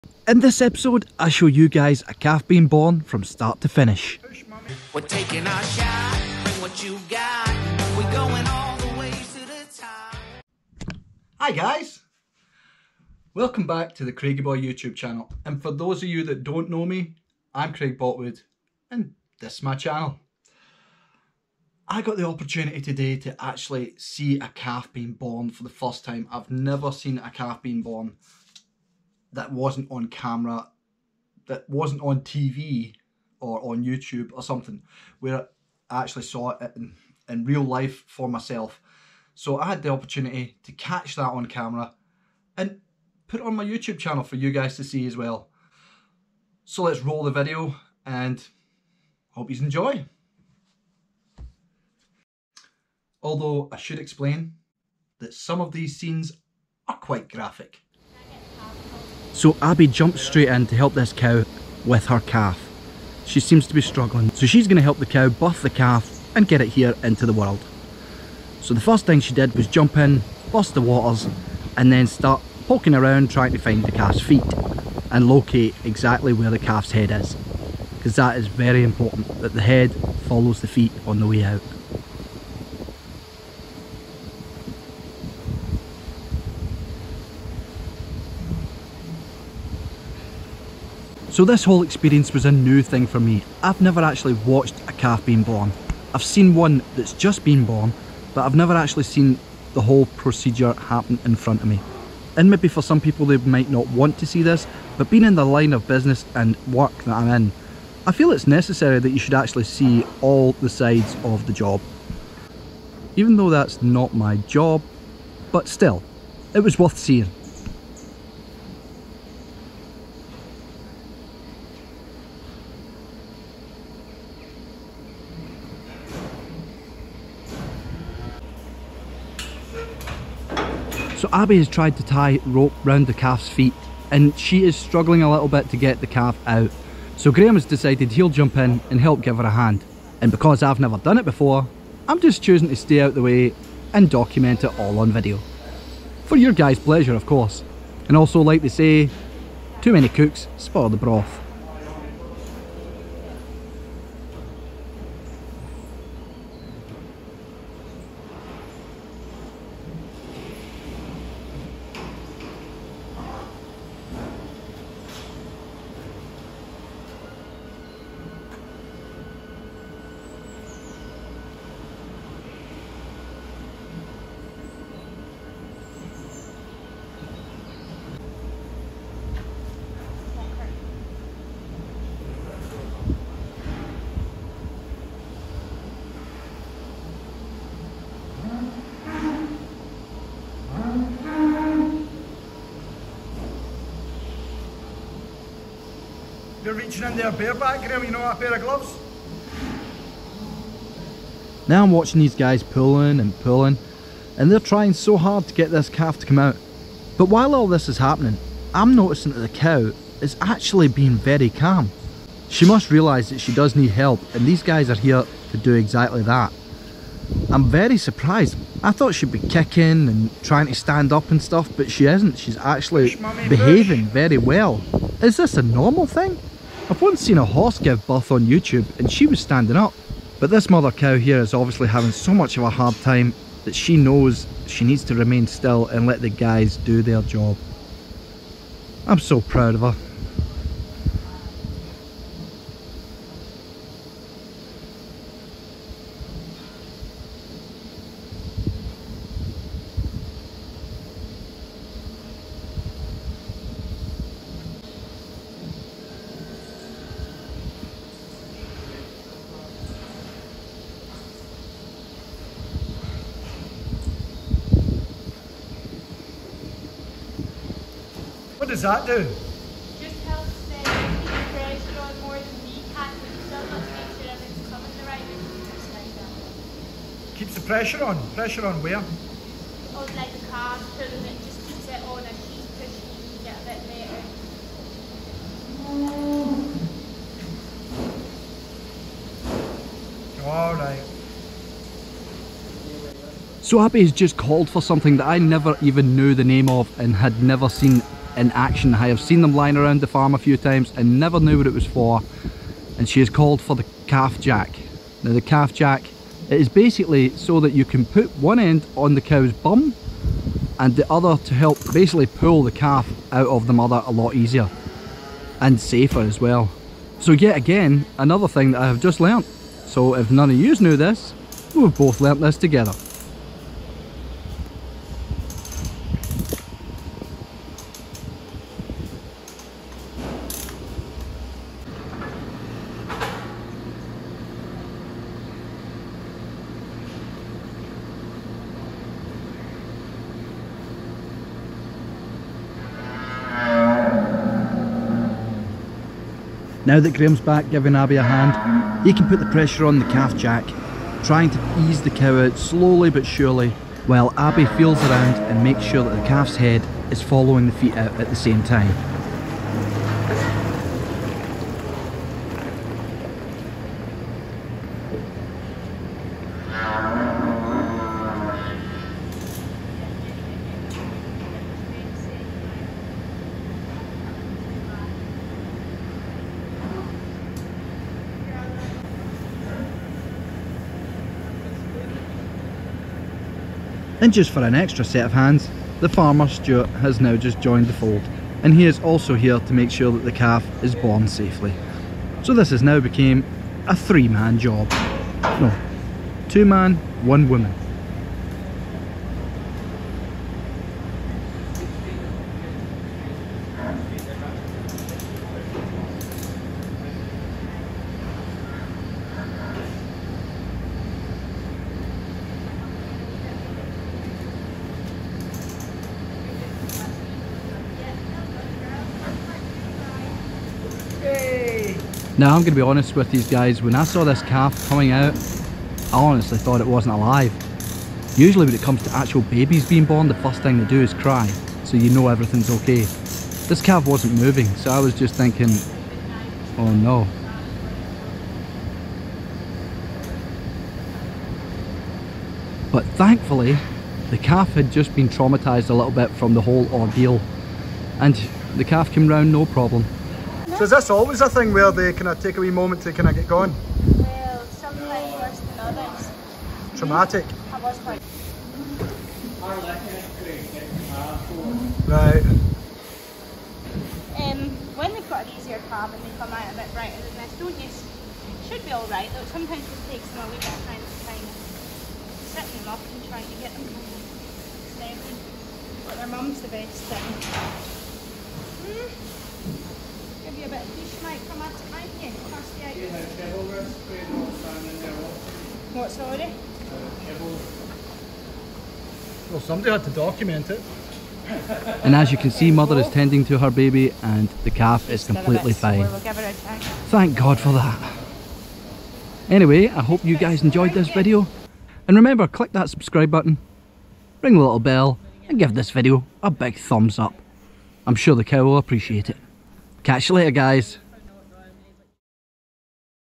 THE HOOF GP HELP'S THIS MOTHER BIRTH HER BABY CALF (FULL VIDEO)
Today's episode has today be one of my favourite post's ever on YouTube. I got to see a calf being born before my very eyes and I managed to get the whole thing on camera. this has it all. a little gore, suspense, fear and the miracle of birth, all in this 8 and a half minute video.